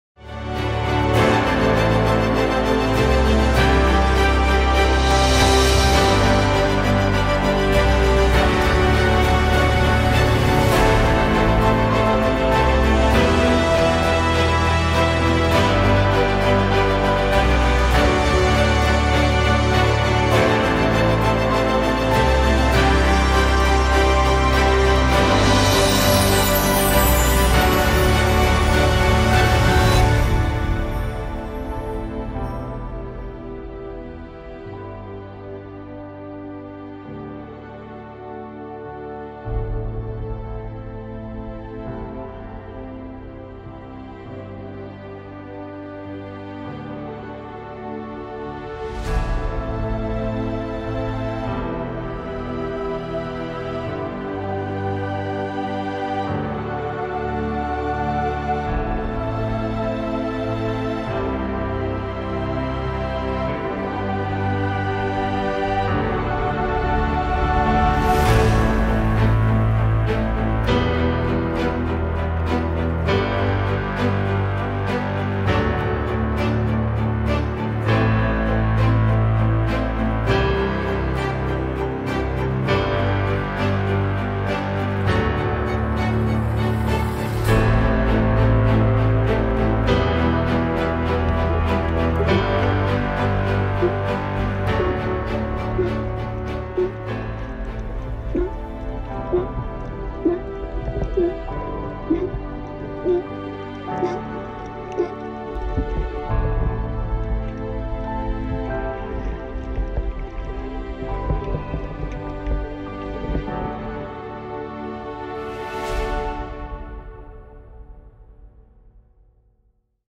1st Place Edited Video entry for the 2018 Southern California Underwater Shootout
Very happy to to announce that I won 1st place Edited Video and 3rd Place Unedited Video categories in the 2018 Bluewater Photo Southern California Underwater Photo and Video competition!  This video is the edited video.  Special thanks to SDExpeditions in La Jolla for taking me out and finding incredible life to shoot for the competition.  Thank you to judges Andy Sallmon, Renee Capozzola, and Scott Gietler for seeing something in my work.